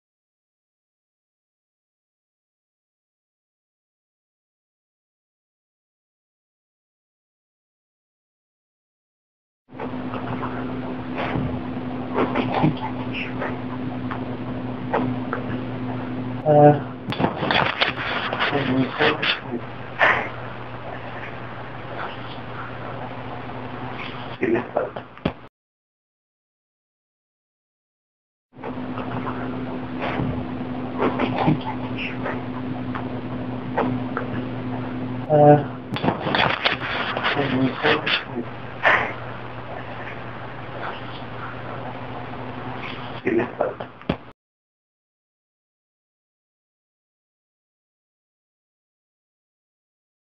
EVP - We Make Contact With Human
EVP Dec 2011 - We Make Contact With Human

This is the tail end of an experiment we were doingusing different frequencies with a sine wave. Although nothing was captured during the actual sine wave experiments.This was captured shortly after we stopped with the sine waves.

All I've done to the audio is increased its volume.